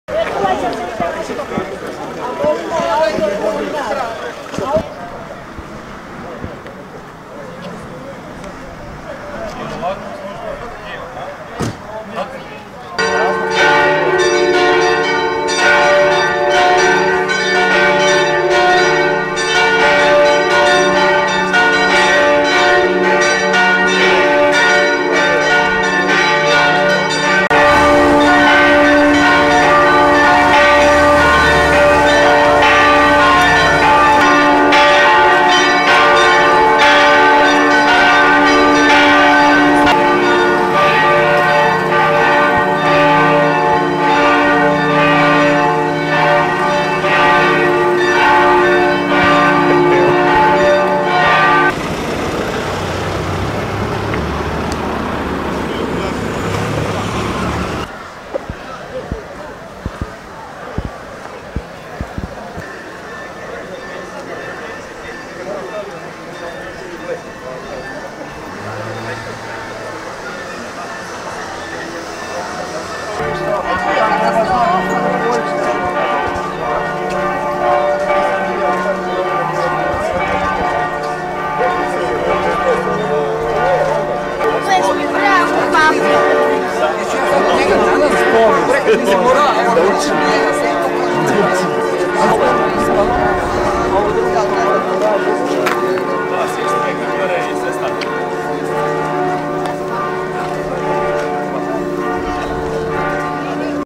Svečana procesija povodom obilježavanja Grada Petrinje i blagdana sv. Lovre
U sklopu obilježavanje Dana Grada Petrinje i blagdana Svetog Lovre, zaštitnika grada, održana je svečana
procesija u središnjem gradskom parku.